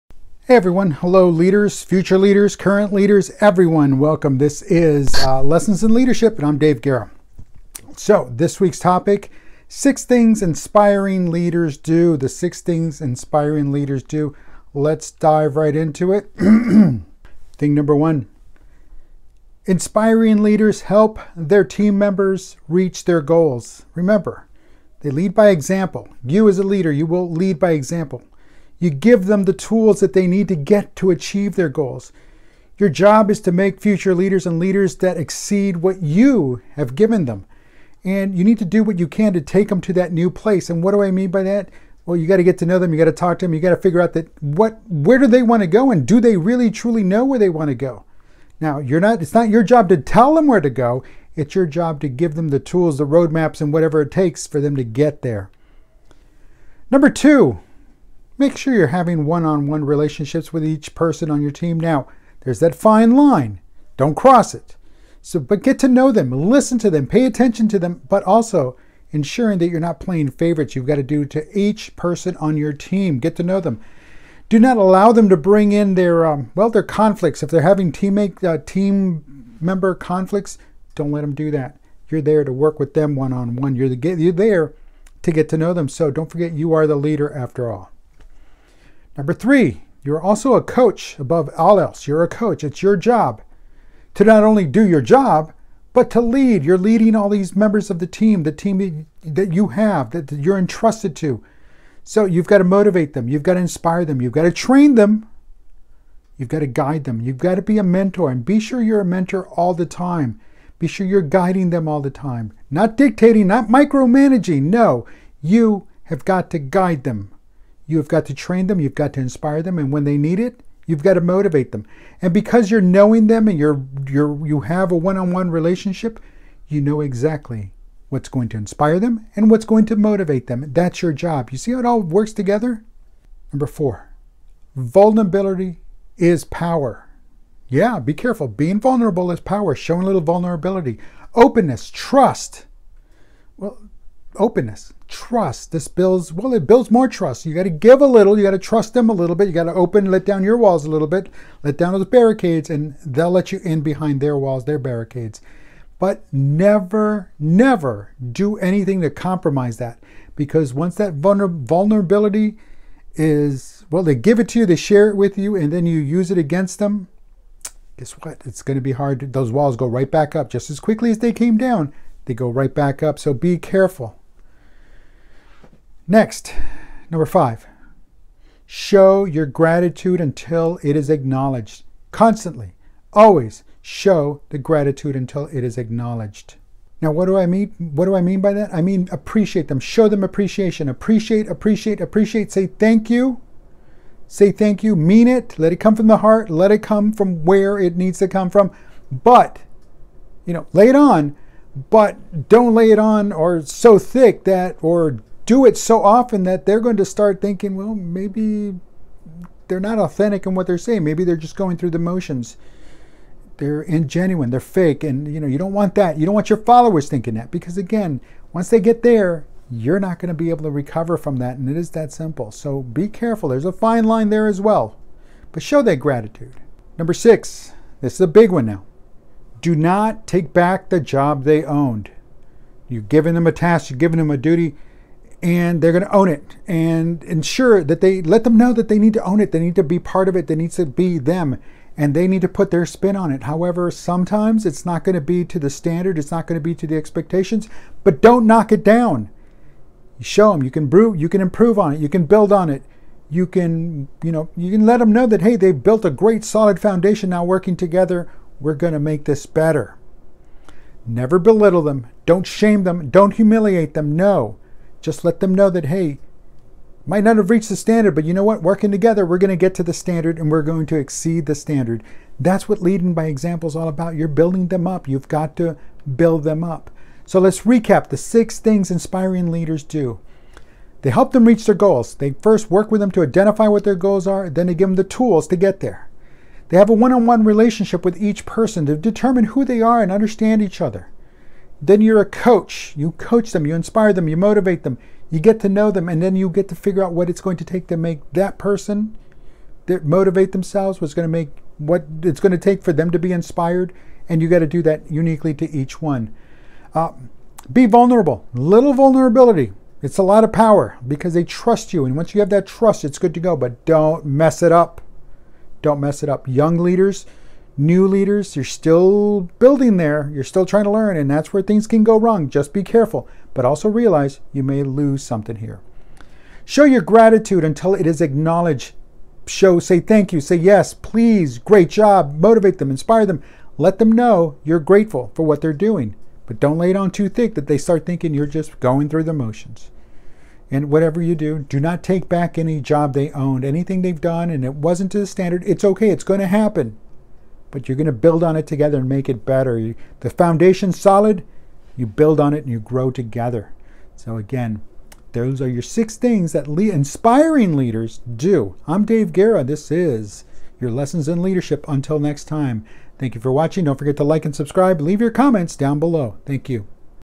6 Things Inspiring Leaders Do - Lessons In Leadership
In this episode, I share with you the Six (6) Things Inspiring Leaders Do.
Being a Leader means being many things. One of those things is that ALL Leaders must motivate and inspire. Here are the six things I believe Leaders must do to be inspiring.
David Guerra is a US Army Veteran, author of the Walking Leader book series, Small Business Starter, Leadership Advocate and Action Taker.